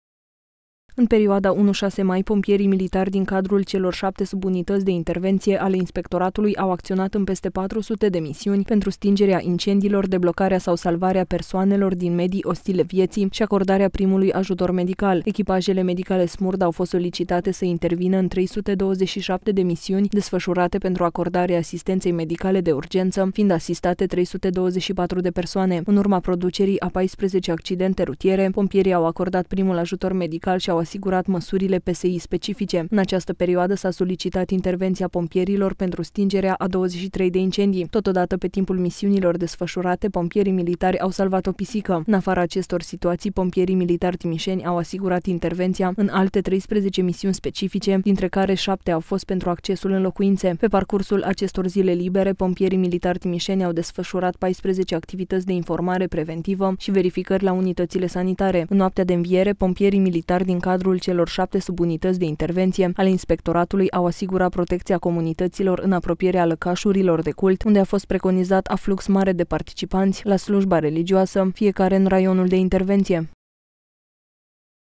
Pompierii au fost la datorie pentru timișeni
În perioada 1-6 mai, pompierii militari din cadrul celor șapte subunități de intervenție ale inspectoratului au acționat în peste 400 misiuni pentru stingerea incendiilor, deblocarea sau salvarea persoanelor din medii ostile vieții și acordarea primului ajutor medical. Echipajele medicale SMURD au fost solicitate să intervină în 327 misiuni desfășurate pentru acordarea asistenței medicale de urgență, fiind asistate 324 de persoane. În urma producerii a 14 accidente rutiere, pompierii au acordat primul ajutor medical și au asigurat măsurile PSI specifice.